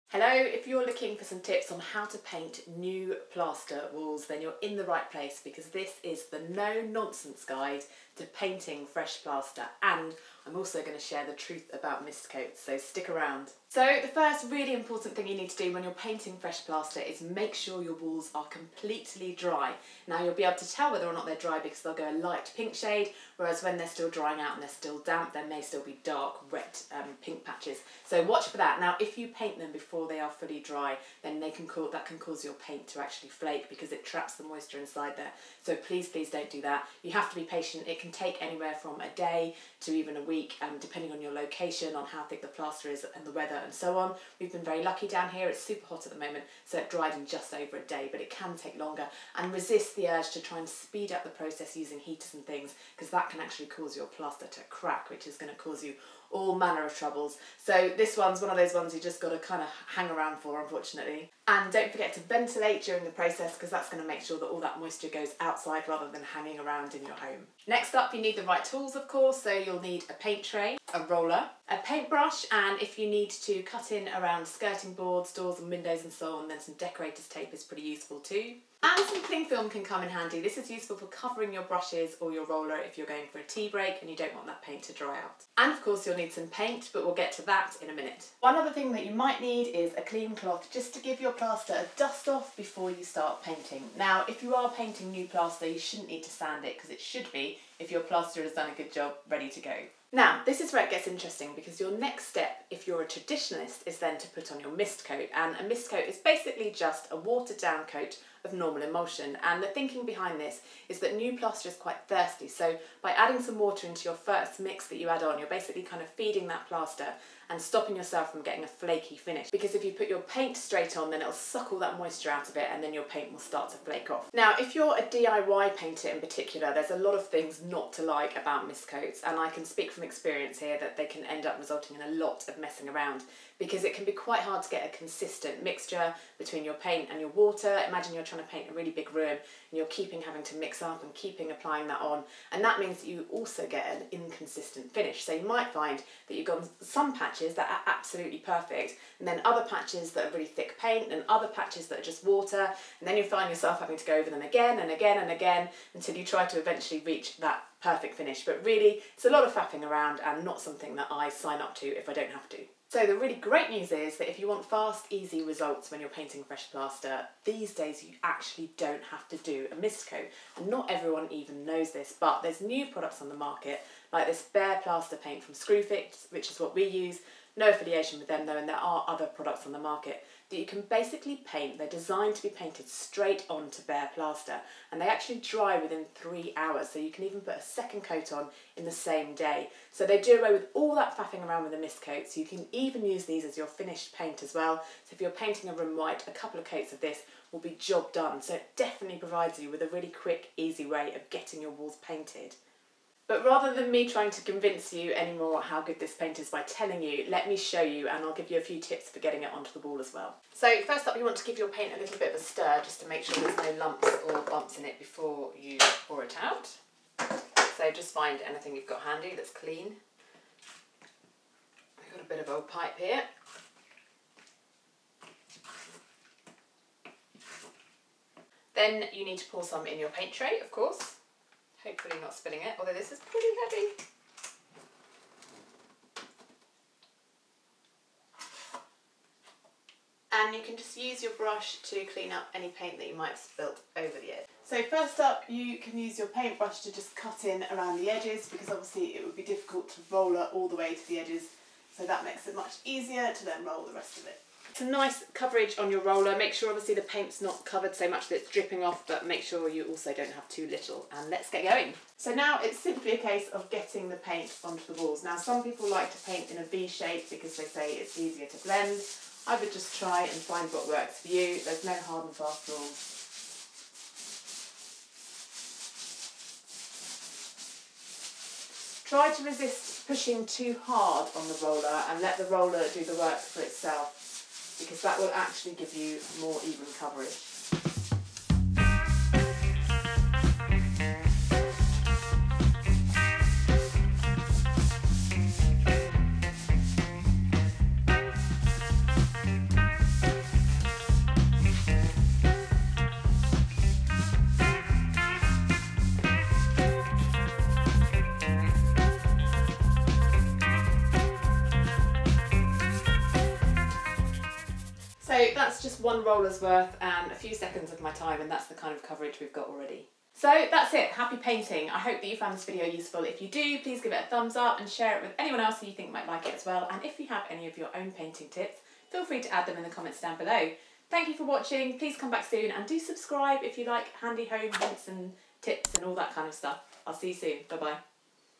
Quick And Easy Tips For Painting Fresh Plaster Plus The Best Paint To Use!
Painting fresh plaster doesn't need to be difficult - you don't even need a mist coat if you make sure you use the best paint for new plaster.

Great news if you have lots of painting to do and not much time.

Products you might need

New plaster paint

Frog tape

Roller set